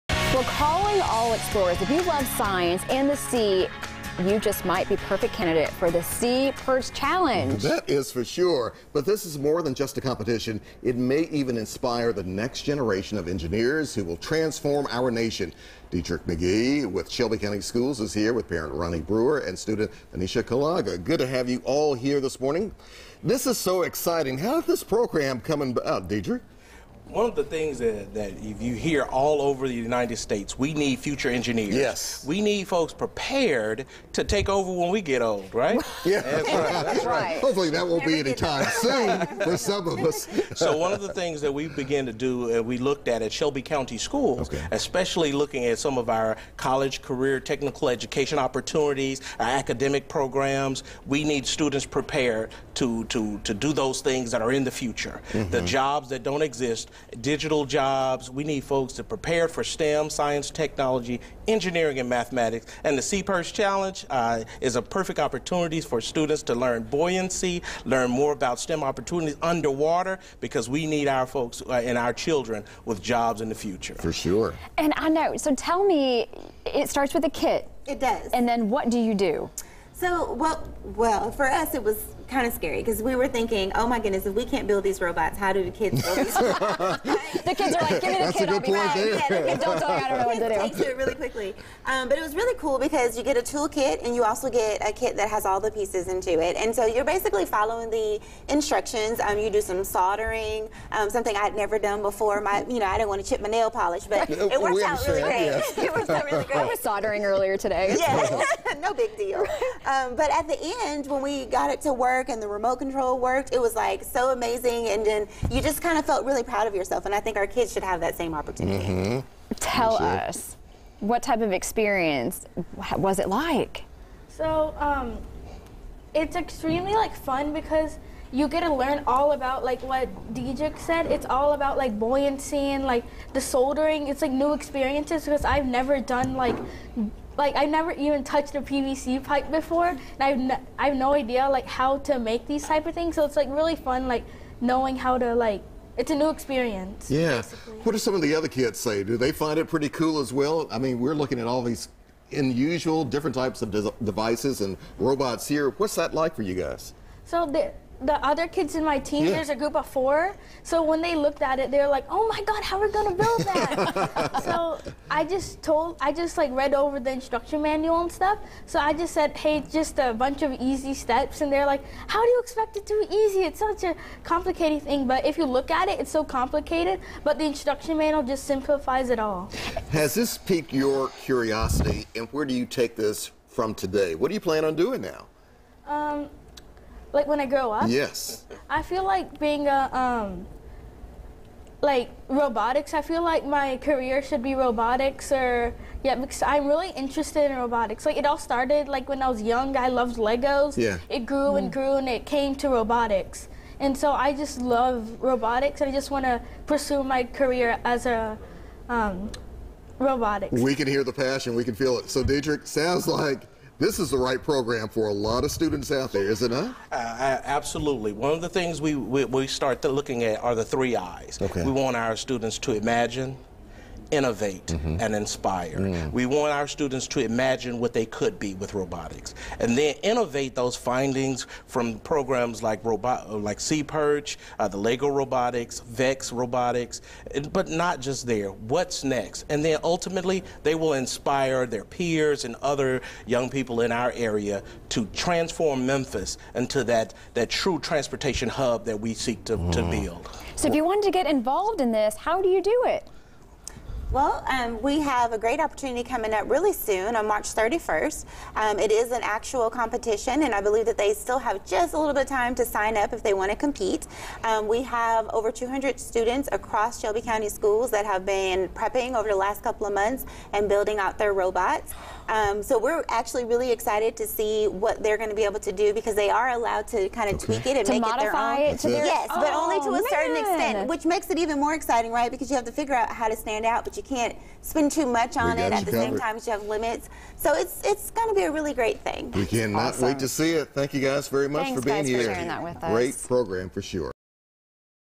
The Memphis SeaPerch Challenge
If you love science and the sea you need to check out the Memphis SeaPerch Challenge- an underwater robotics competition that could inspire the next generation of engineers.